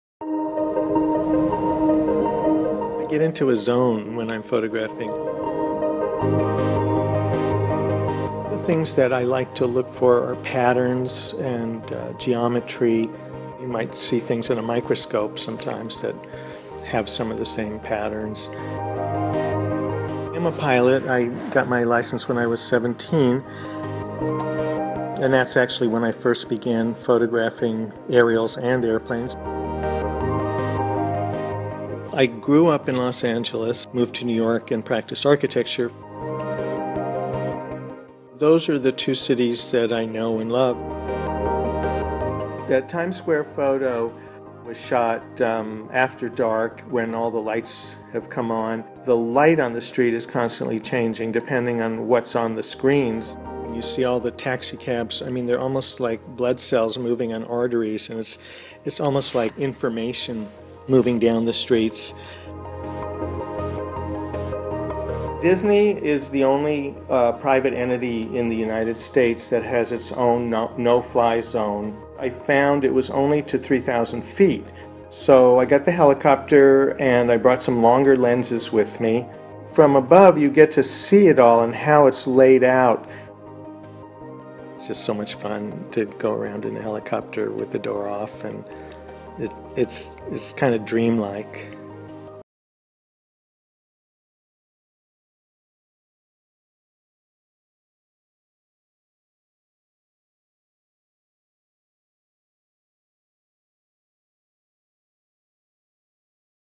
Stunning aerial photos capture US cities from above - BBC News
Photographer Jeffrey Milsten's recent book LA NY captures aerial photos from helicopter rides across Los Angeles and New York.
Video by Arijeta Lajka